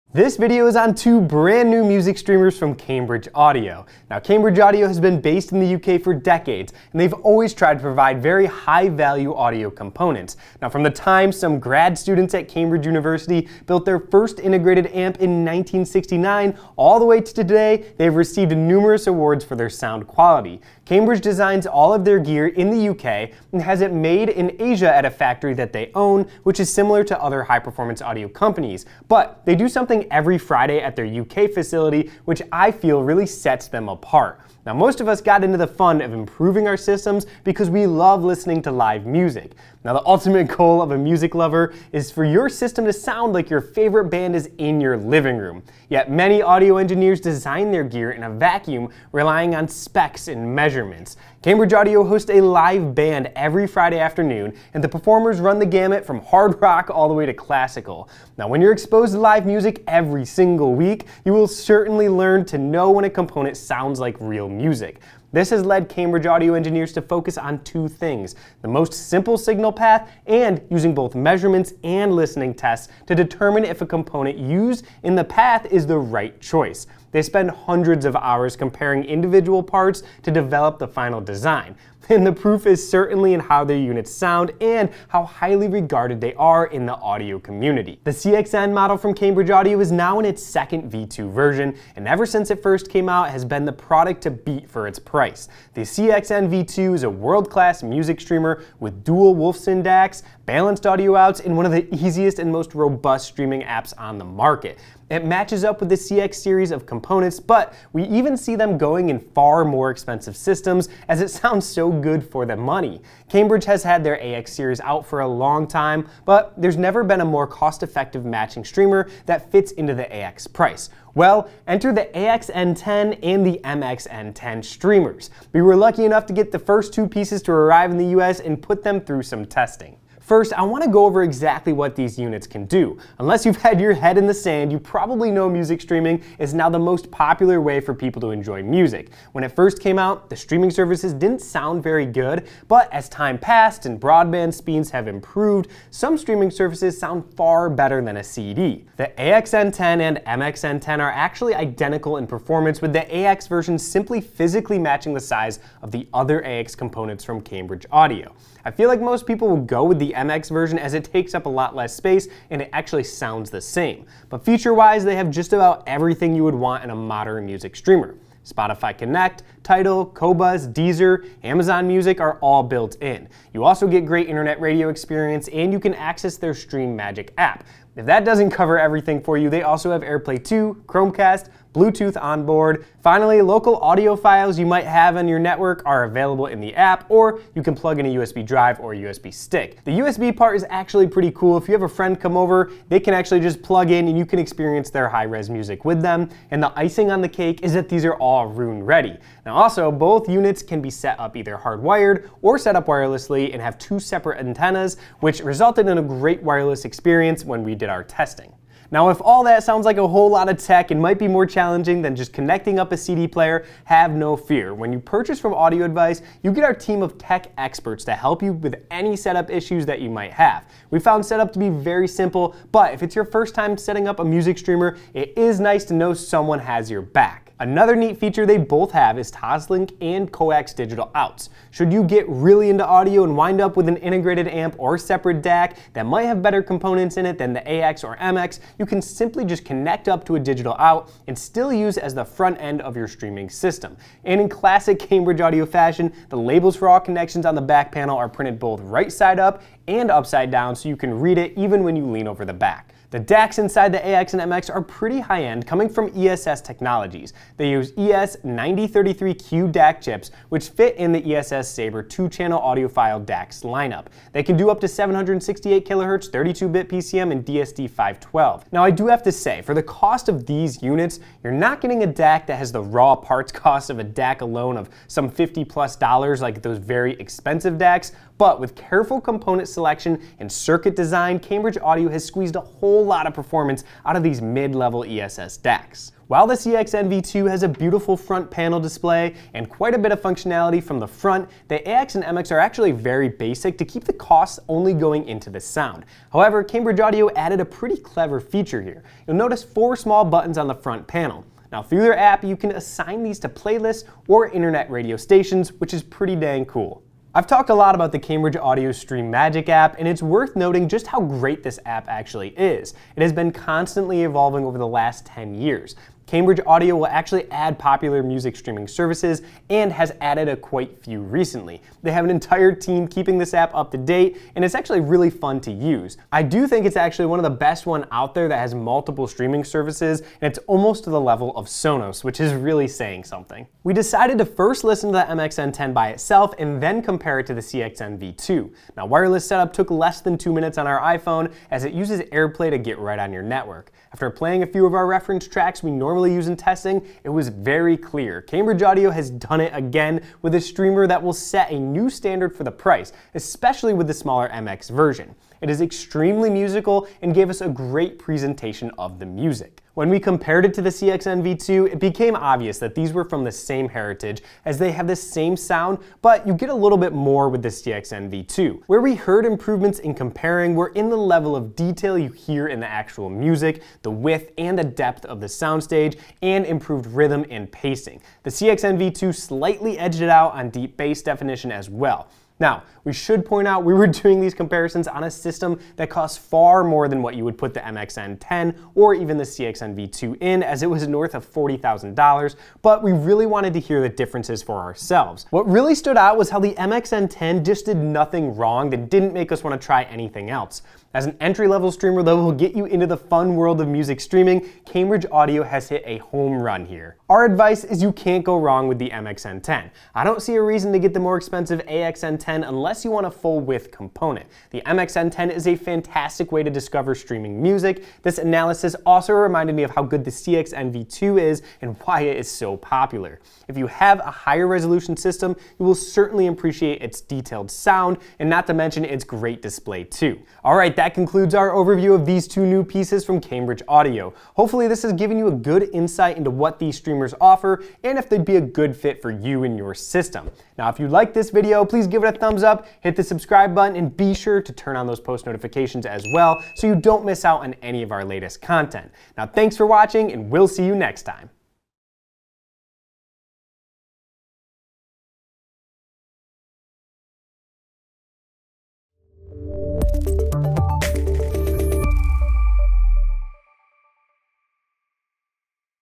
Cambridge Audio AXN10 & MXN10 Network Streamers Overview
Although the new Cambridge AX and MX streamers don't have onboard MQA decoding, there is a solution for a customer really wanting it. You could pair one of the new streamers with the DAC Magic 200 and use the DAC Magic to decode the signal and unfold MQA. Just connect them together with a coax digital cable. Not only will you get a better DAC chipset producing the sound, but they will get MQA as well. The MX would be a perfect pairing since it and the DAC magic are about identical in size.

We have always loved Cambridge Audio products for their value and sound that just draws you into the music. Following up on their super successful CXN v2 music streamer, Cambridge has released two new streaming models, the MXN10 & AXN10, that set a new standard for price and performance while still providing that classic Cambridge Audio Sound. Both designs offer the same set of features making it easy to get into the fun world of high-resolution music streaming. The full-width AXN10 brings the world of hi-res music to much-loved hi-fi set-ups and other audio systems. The MXN10's half-width footprint offers greater flexibility of placement.

0:00 What Sets Cambridge Apart
1:53 New Streamers
2:10 What They're Capable Of
3:28 We're Here To Help
3:47 Great Features
4:12 DACs
4:46 Looks
5:34 Performance
6:47 Overall Recommendation